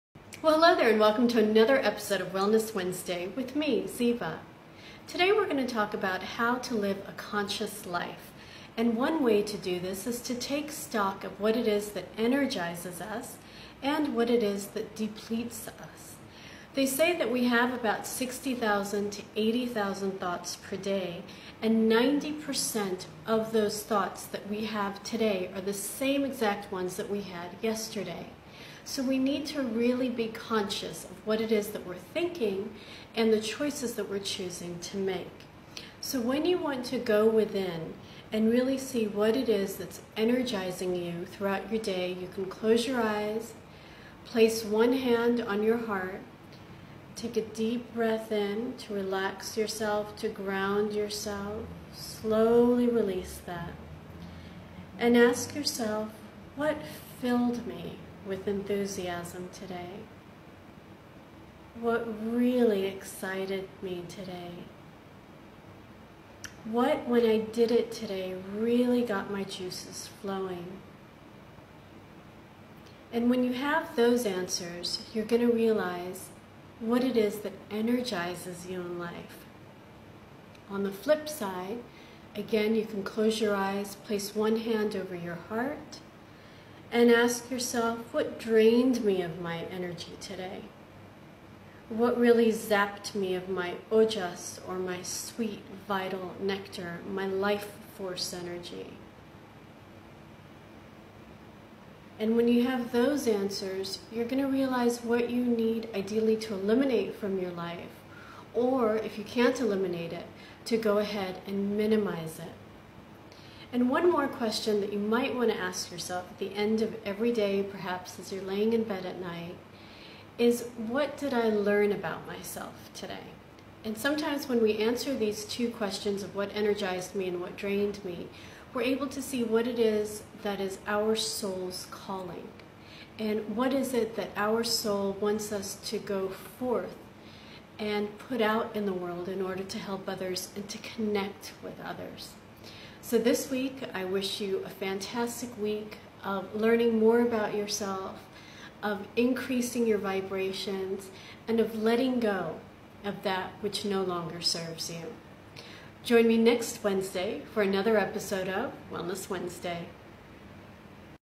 Are You Living a Conscious Life? — Wellness Wednesday #14
In this week's Wellness Wednesday discussion we talk about how to live a conscious life.

Make sure to like this video and share it with someone who can benefit from it.